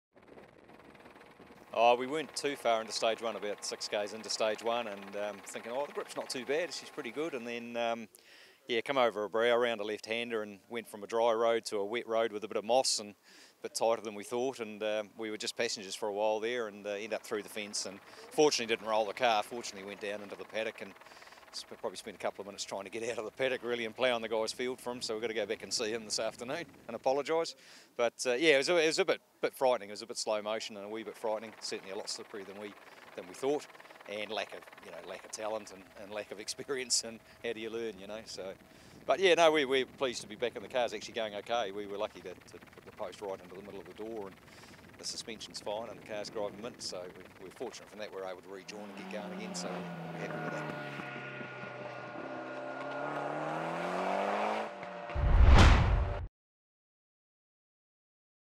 Early Excursion for Andrew Simms - Targa Rotorua 2016
Andrew Simms flew in by chopper to make the start of SS1 this morning and then flew straight off into the greenery but kept it upright and got back on the road - top effort!